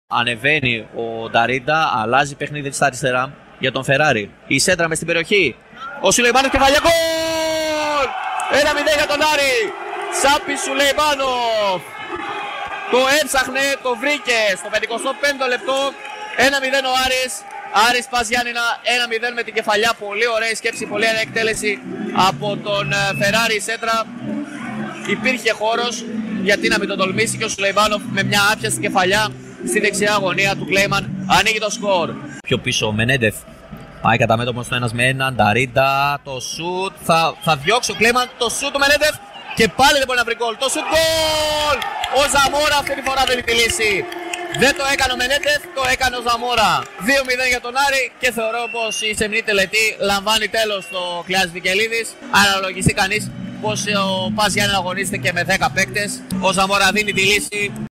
LIBERO 107,4 | Άρης - ΠΑΣ Γιάννινα | Τα ΓΚΟΛ σε περιγραφή Κωνσταντίνου Τσίπτσιου
Ακούστε τα γκολ της νίκης του Άρη επί του ΠΑΣ Γιάννινα για την 9η αγωνιστική της Super League σε περιγραφή του Τσίπτσιου Κωνσταντίνου.